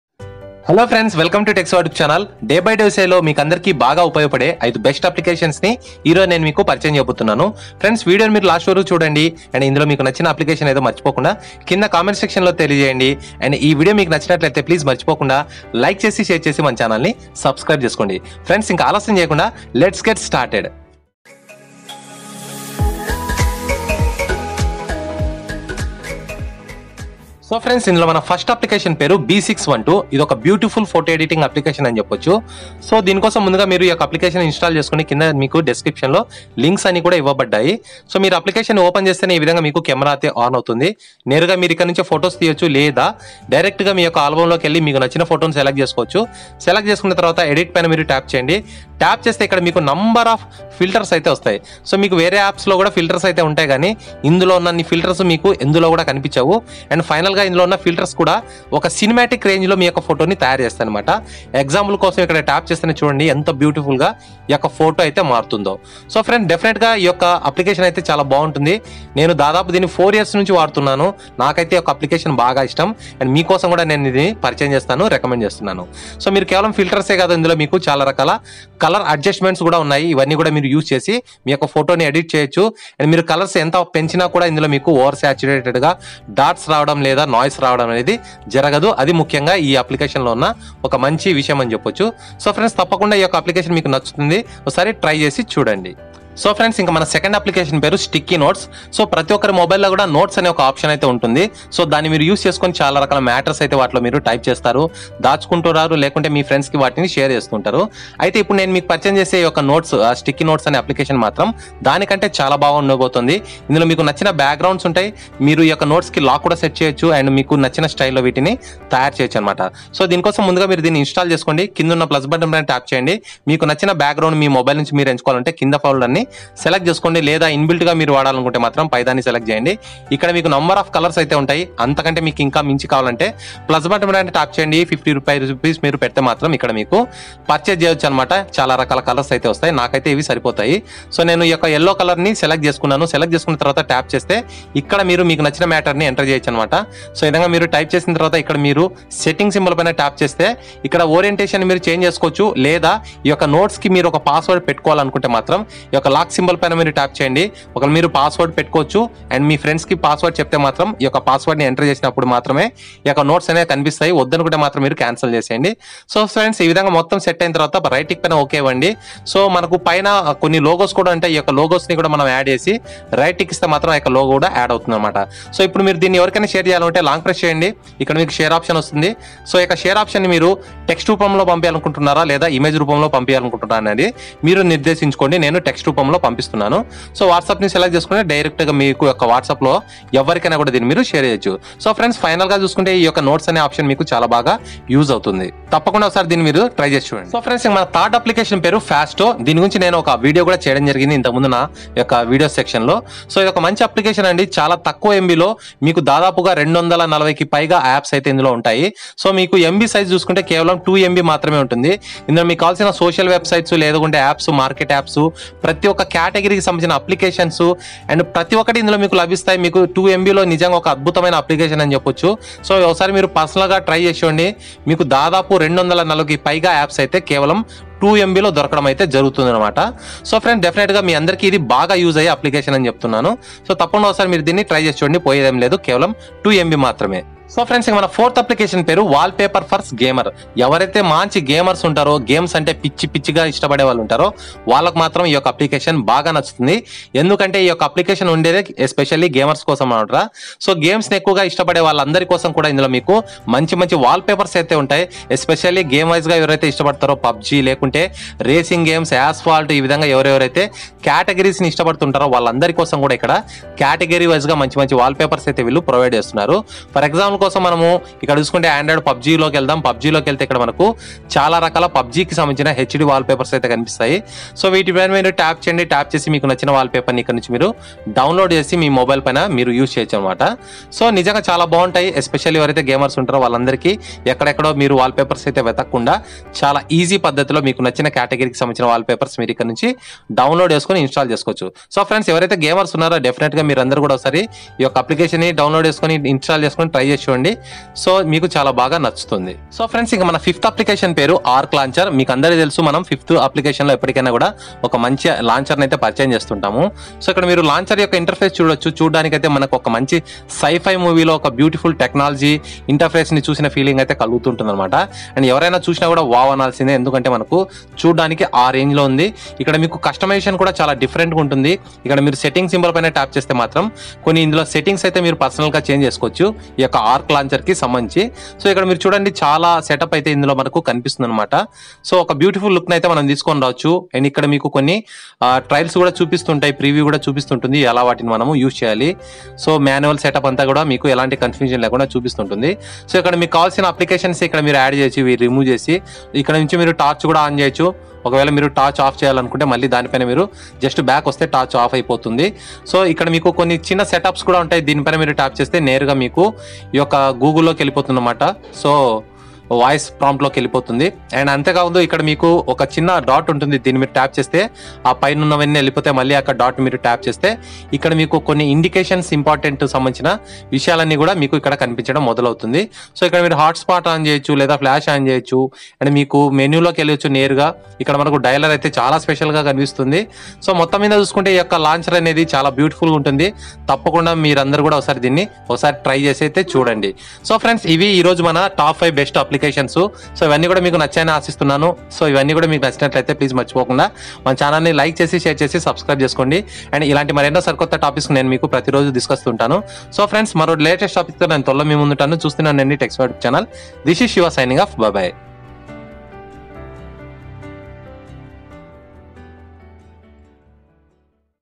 5 అదిరిపోయే మంచి Apps | 5 Fabulous Apps To Try Now | Best Interface & Photo Editing Apps | Tech Siva
5 అదిరిపోయే మంచి Apps. In this video, I show you some of the Top 5 Fabulous Apps To Try Now. Check out this full video to know more about Best Interface Apps & Photo Editing Apps to edit and post them special.

App links here:-